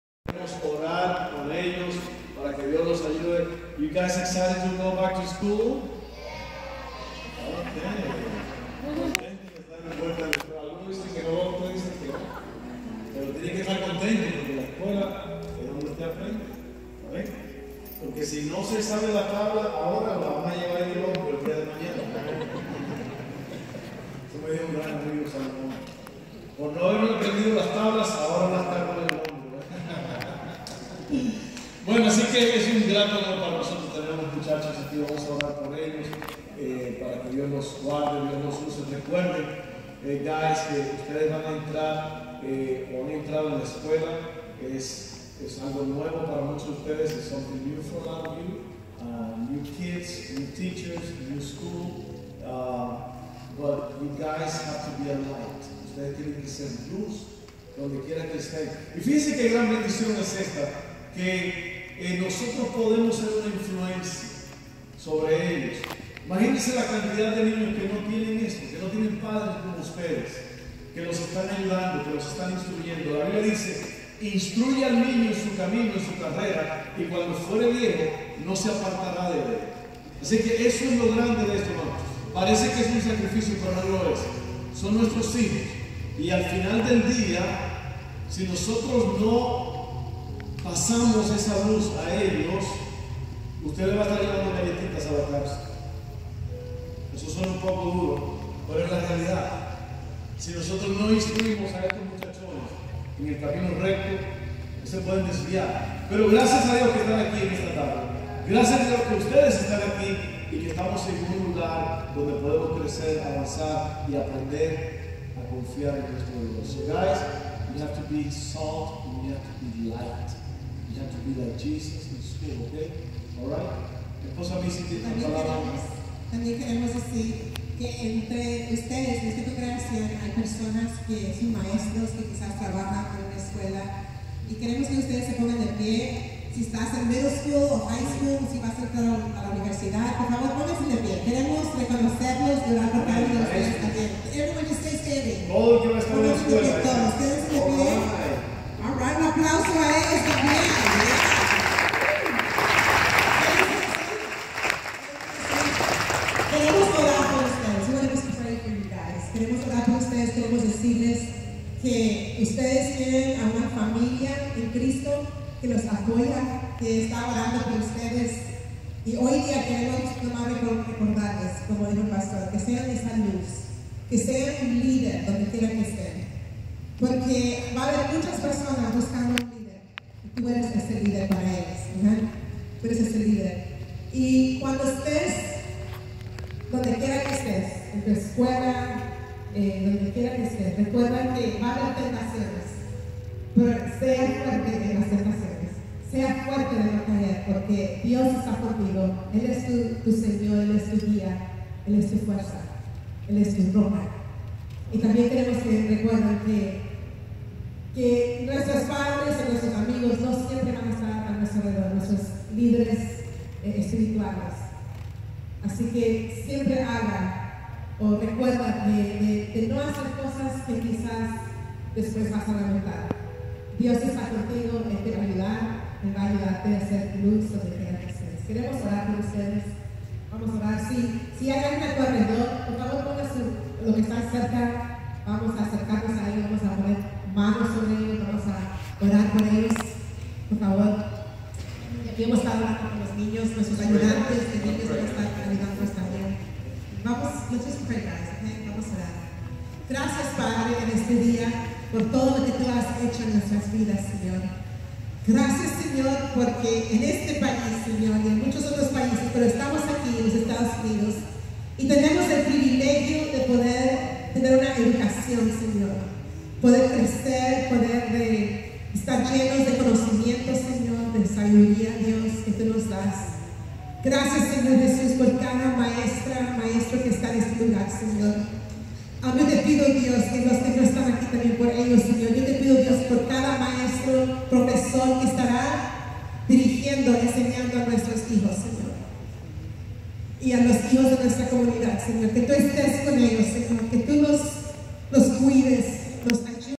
Oracion de regreso a clases 2017
En Iglesia Lugar de Sanidad oramos por los estudiantes en cada regreso a clases para dedicarlos y que sean luz a donde quiera que vallan.  Si usted no lo hace o no hacen esto en su iglesia es buena idea para practicarla.